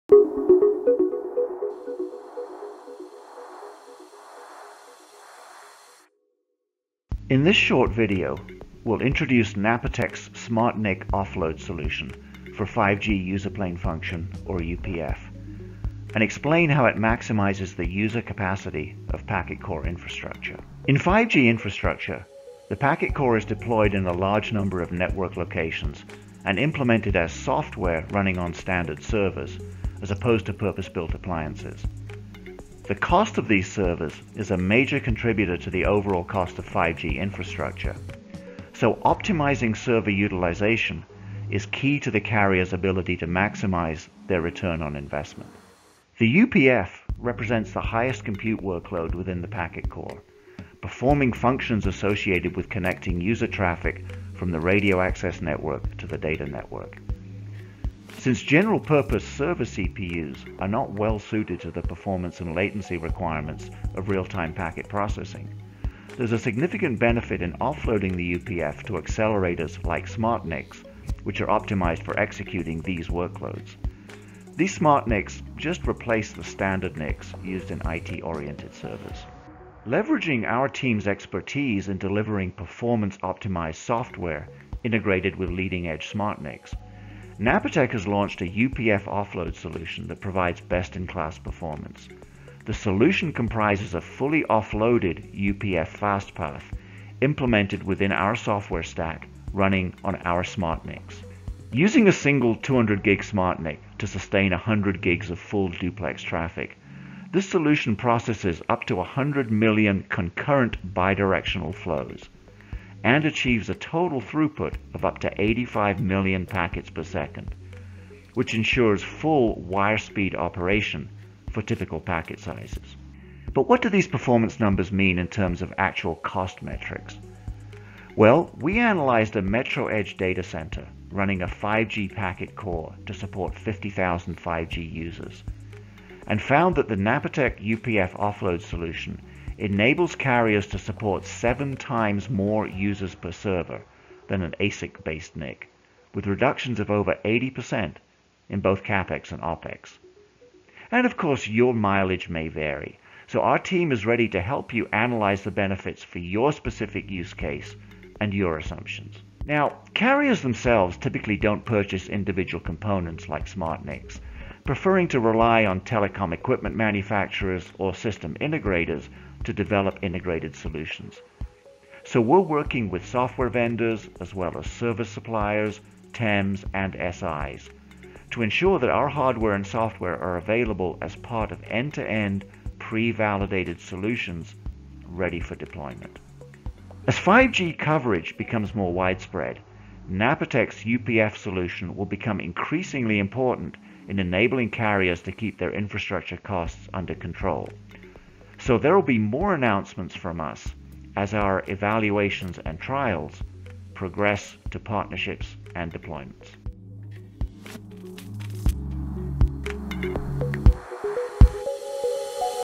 Napatech SmartNIC Offload Solution for 5G User Plane Function
Napatech’s 5G UPF Offload Solution to minimizing 5G infrastructure costs presented by Napatech Senior Director Business Development Charlie Ashton.